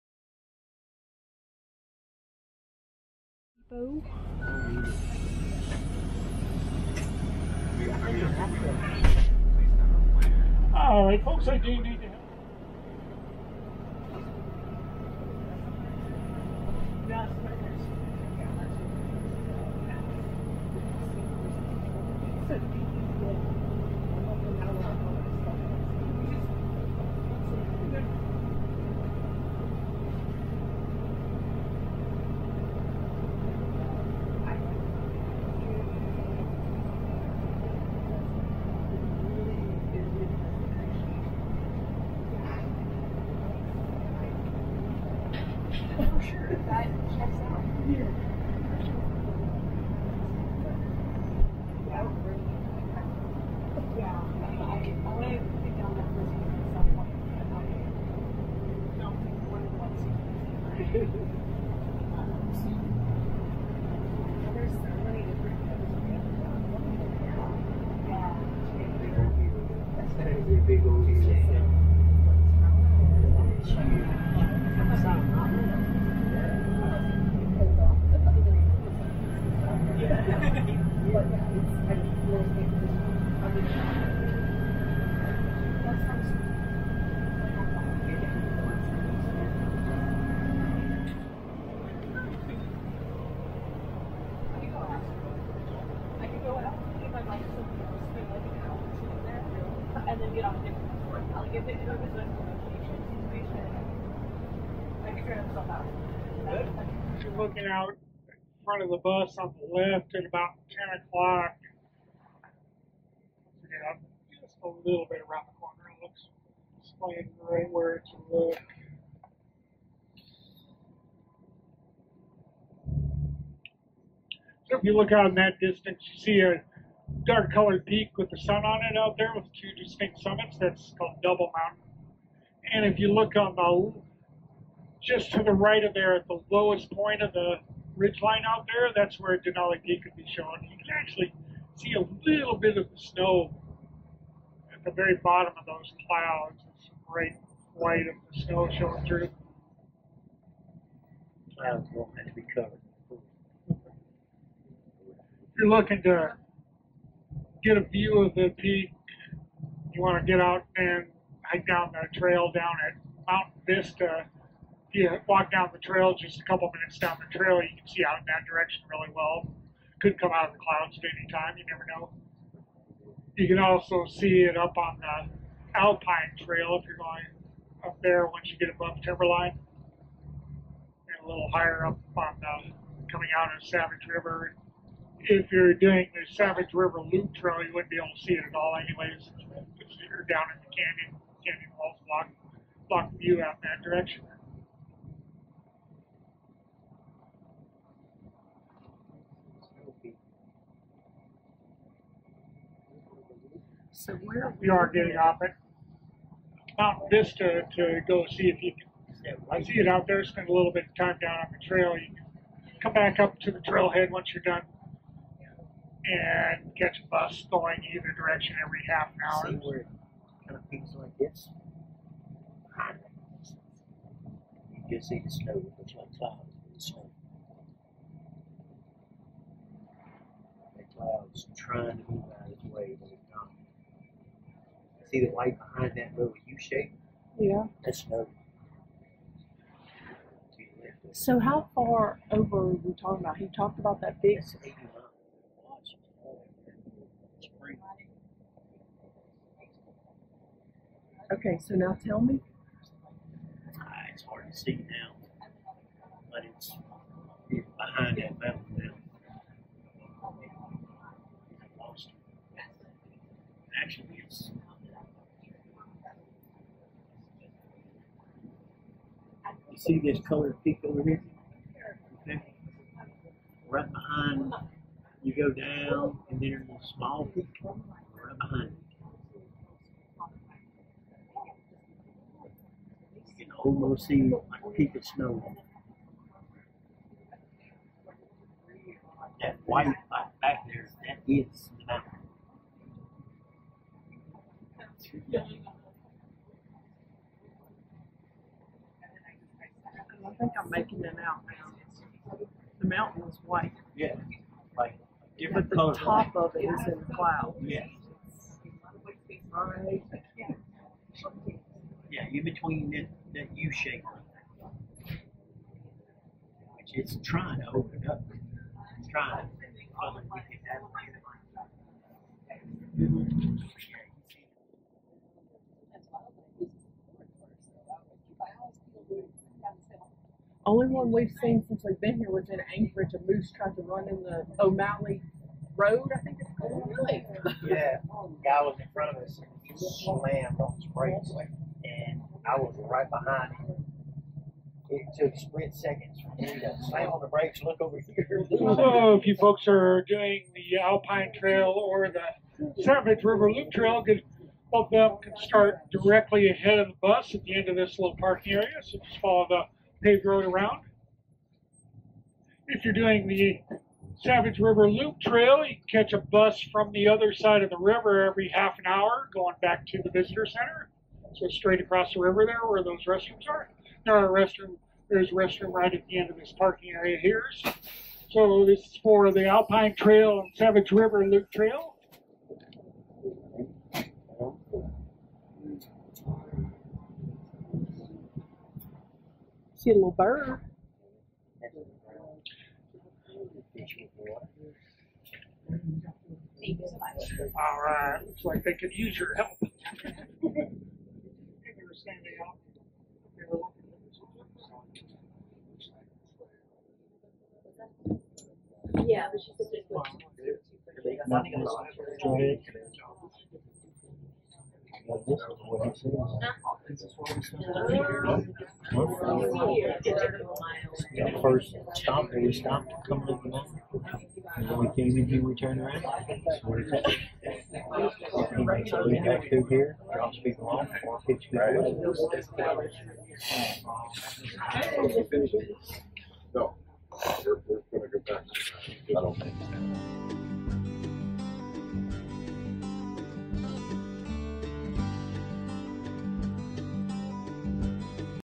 Thrilling Ride On The Savage River Shuttle In Denali National Park, Alaska!
We took a 2 hour ride on the Denali National Park Savage River Run shuttle. This got us a little into the park and gave us a lot of beautiful views.  We were not able to see Denali's peak on the shuttle ride, because it was covered by clouds, but we still enjoyed the other sights.